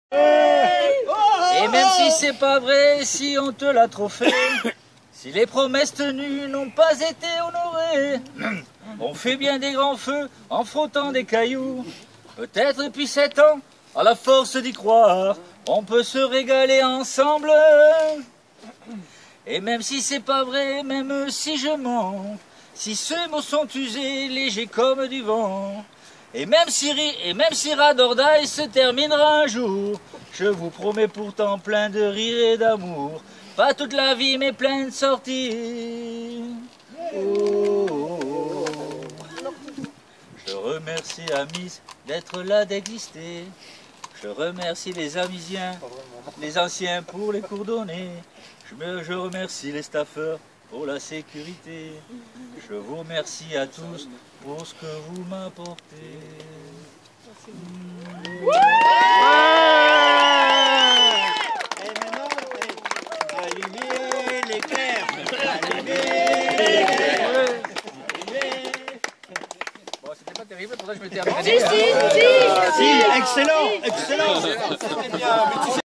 Annif roller Daniel
Pour son anniversaire fêté au ride or die en 2014, Daniel nous chante une chanson créée par lui, pour les rider, sur une musique de Johnny Halliday